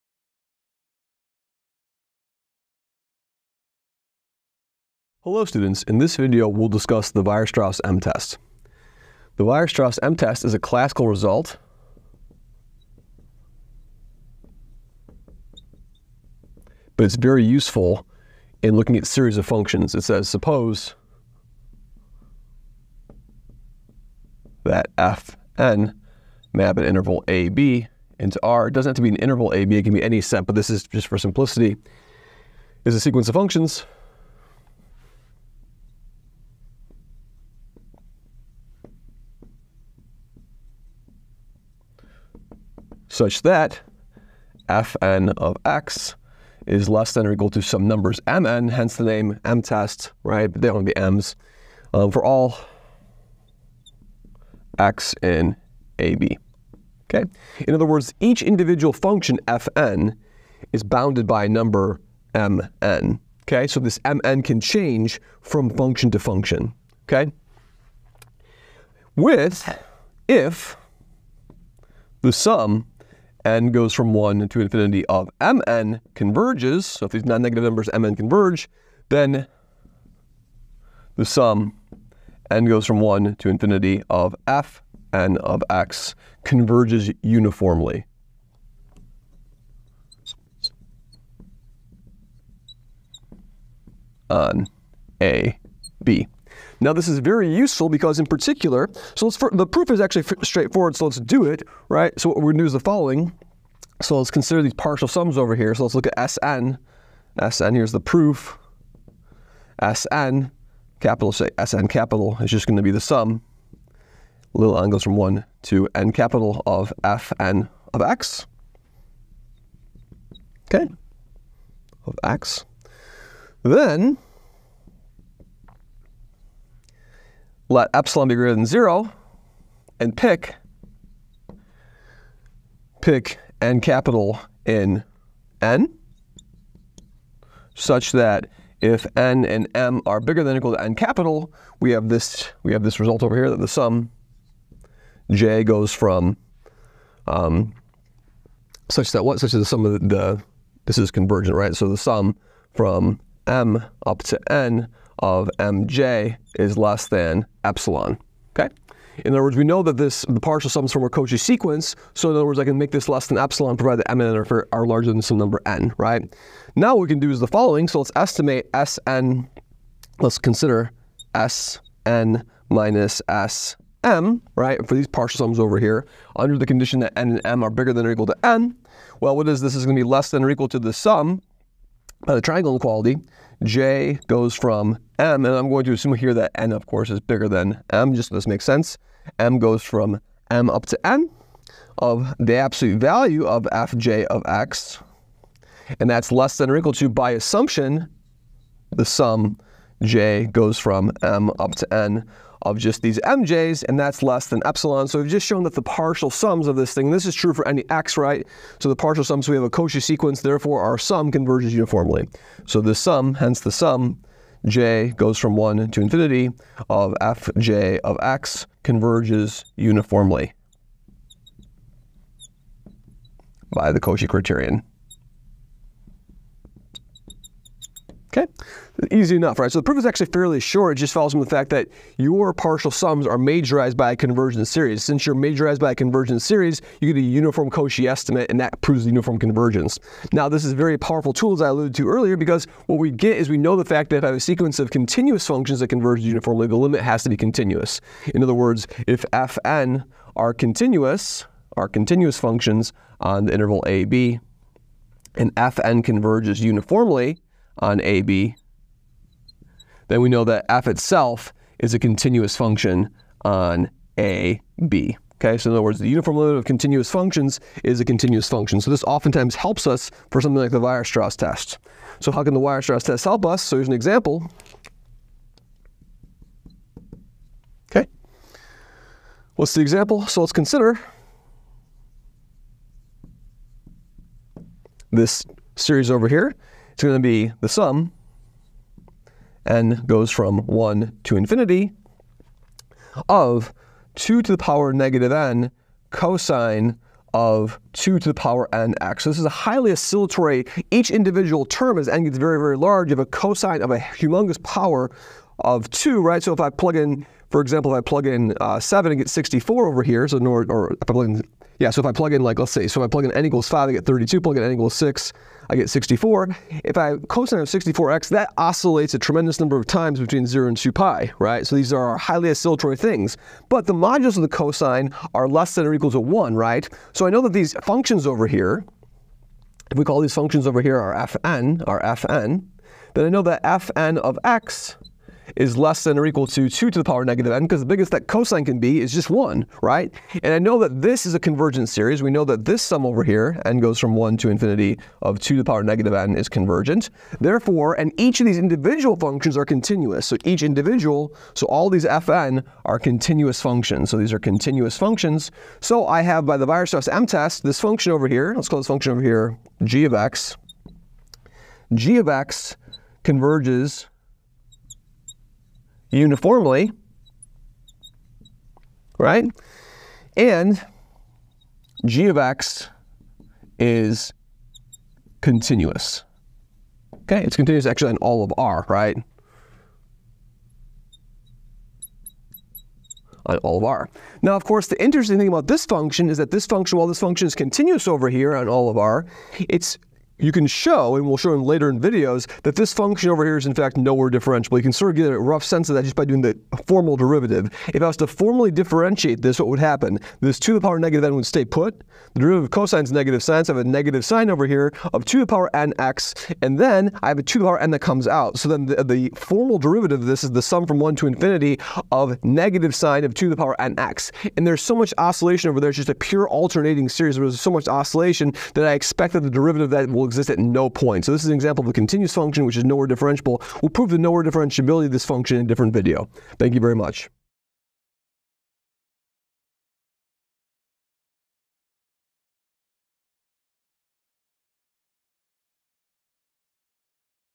The Weierstrass M- Test
We prove the Weierstrass M-Test which states that if a sequence of functions is majorized by a non-negative sequence which is summable, then the sum of the f_n converges uniformly. In particular, if a sequence of continuous functions is bounded by a non-negative sequence which is summable, then the infinite sum will be continuous.